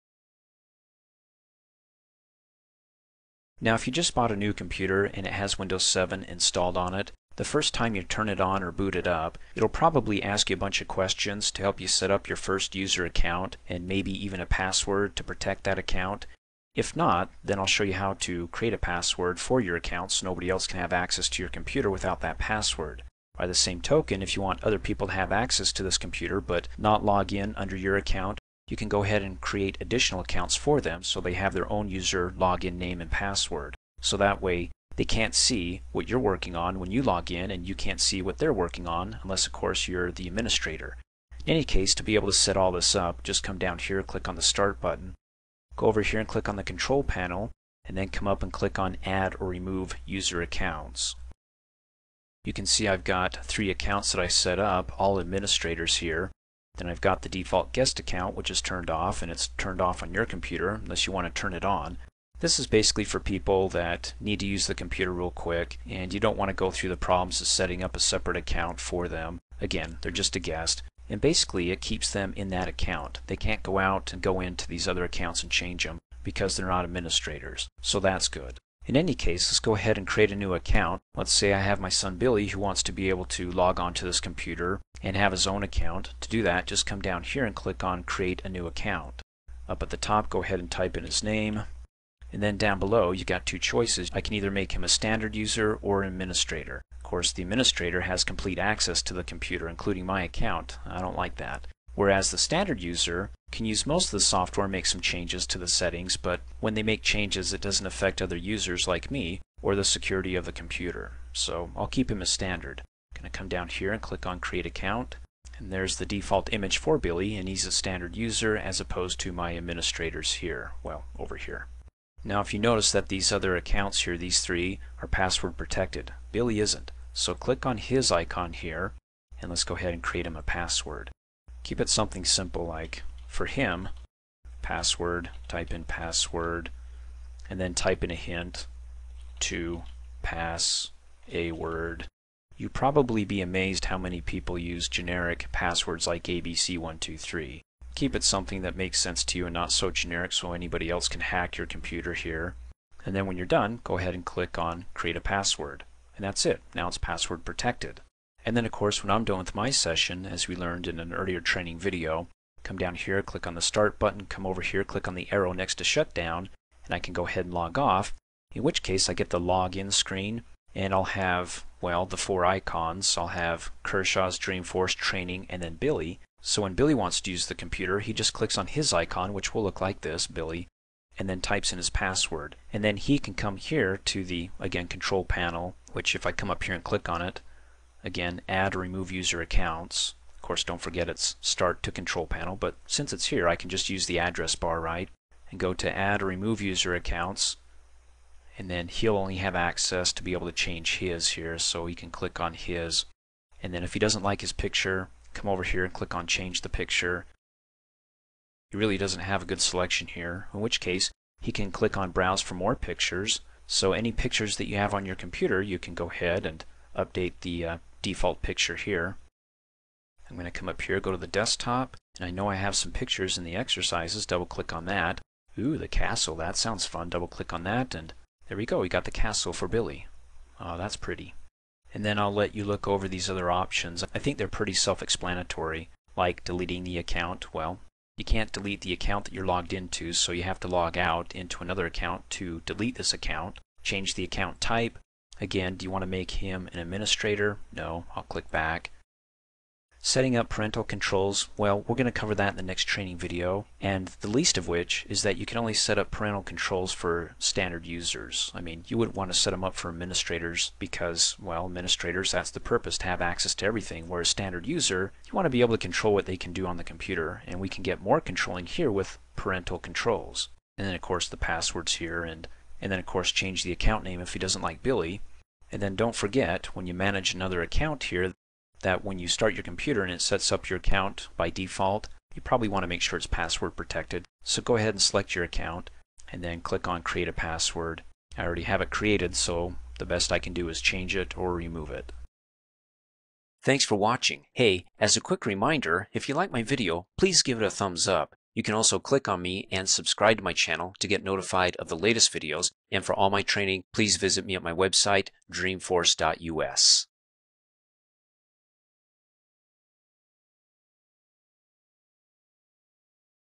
Windows 7: How To Create A User Logon Account With Standard Or Administrator Privileges
Windows 7 training video on how to create additional, separate long on accounts for additional users on the same computer.  You can set each user up with a password and a level of access privileges including Standard that the user can use most software and change system settings that don't affect other users, or the security of the computer; or Administrator who has full control.  As well as how to remove, or delete a User's account.